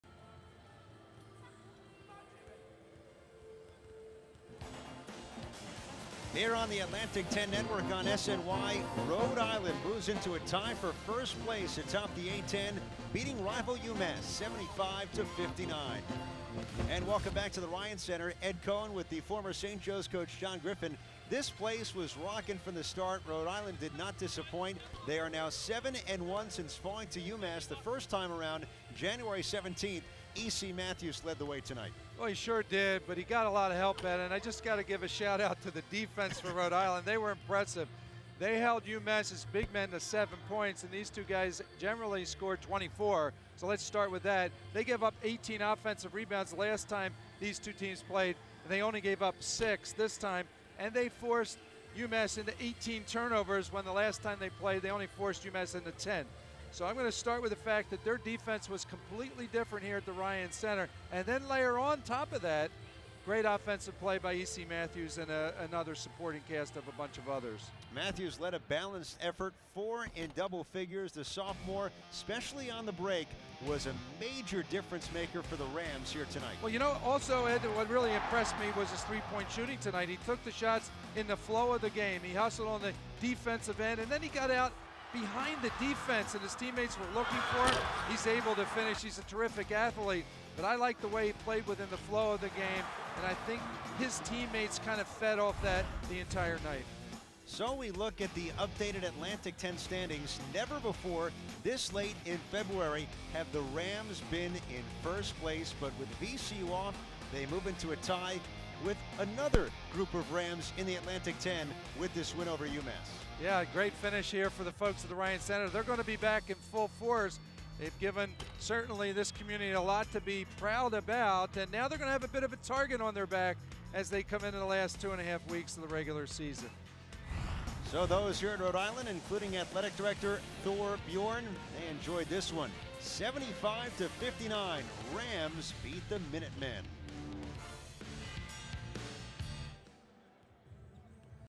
UMass-Rhode Island Web Hit
A postgame recap of Wednesday's UMass-Rhode Island men's basketball game